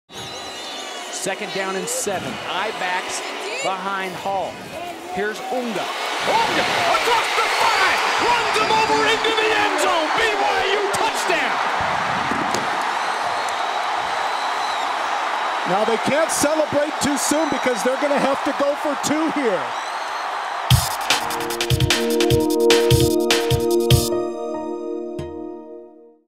BYU Utah 2007 Unga touchdown
In 2007 Harvey Unga rushed for over 1000 yards in just 11 games, with this mark reached in the second quarter against their in-state rivals, Utah, on November 24, 2007. He was named the MWC Freshman of the Year. BYU went on to win this game 17 to 10.